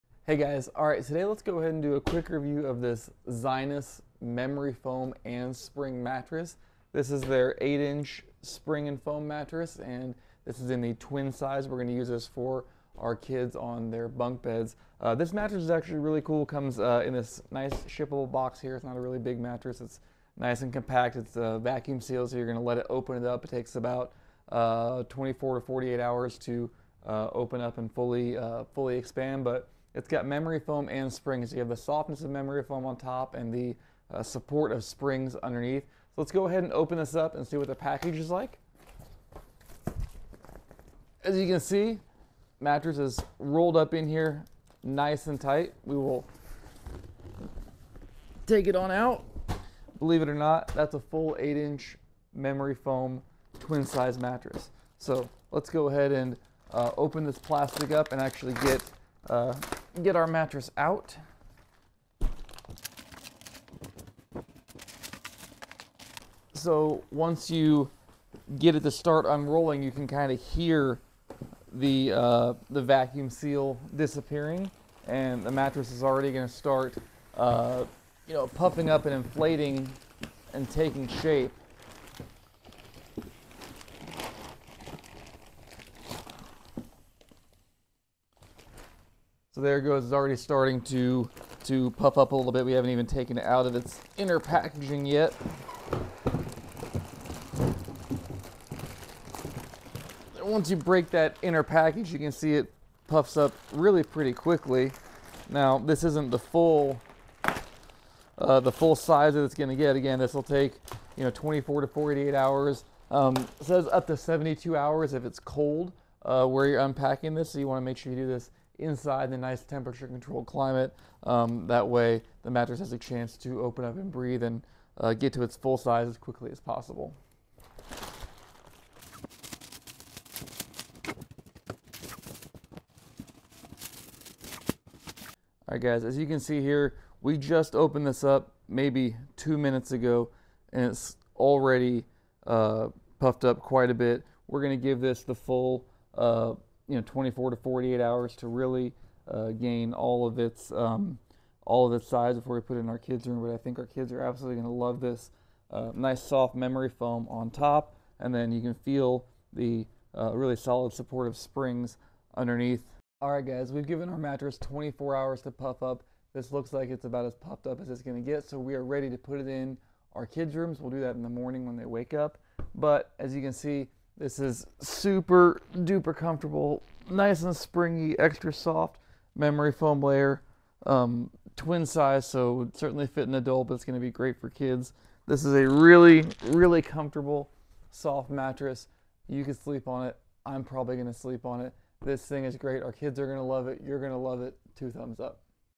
Zinus MEMORY FOAM MATTRESS FULL REVIEW plus COMFORT TEST!
Introducing the Zinus 8 Inch Foam and Spring Mattress - the perfect combination of comfort and support for a restful night's sleep.

This mattress is designed with a durable base of supportive steel coils, which are encased in high-density foam to provide superior motion isolation and reduce noise. On top of this sturdy foundation is a layer of comfortable foam that contours to your body, providing pressure relief and cushioning.

Not only is this mattress comfortable, but it's also safe and environmentally friendly. The foam used in this mattress is CertiPUR-US certified, which means it's made without harmful chemicals and meets rigorous standards for content, emissions, and durability.

At only 8 inches thick, this mattress is perfect for those who want a low-profile bed or need to accommodate a bunk bed or trundle. And with its convenient mattress-in-a-box design, setting up your new bed is as easy as unrolling the mattress and letting it expand.

Upgrade your sleep with the Zinus 8 Inch Foam and Spring Mattress, available now in Twin size.